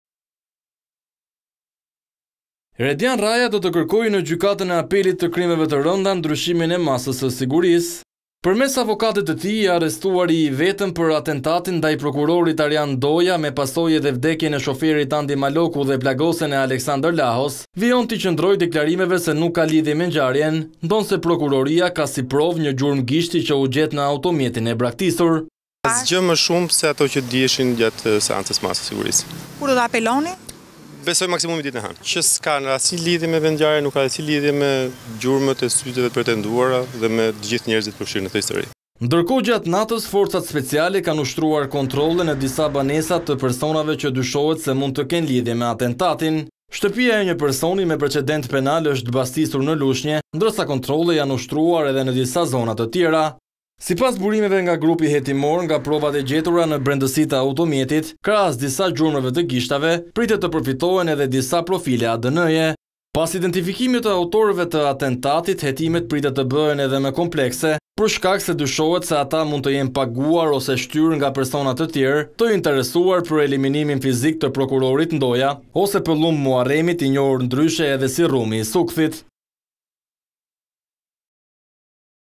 Redjan Rraja kërkon lirinë, ankimon vendimin në Apel - News, Lajme - Vizion Plus
Redjan Rraja ka ankimuar në Apel masën e arrestit me burg të dhënë nga Gjykata për Krime të Rënda.34-vjeçari akuzohet nga prokuroria si një nga atentatorët e prokurorit Arjan Ndoja, që mbeti i plagosur më 1 nëntor 2019 në mbikalimin e Shkozetit, së bashku me Landi Muharremin, ndërsa shoferi Andi Maloku ndërroi jetë dje në spitalin e Traumës.Ankimimi është paraqitur nga avokati i 34 vjeçarit, i cili kërkon lirinë me argumentin se është I pafajshëm.Në makinën që autorët braktisën pas atentatit të cilin autorët nk mundën ta digjinin, uniformat blu krahas provave të tjera gjetën dhe një palë syze mbi të cilat pati gjurmë gishtërinjsh që korrespondojnë me ato të Redjan Rrajës.Gjurmë që nuk janë pranuar nga ana e Redjan Rrajës, I cili ka mohuar të ketë lidhje me atentatin.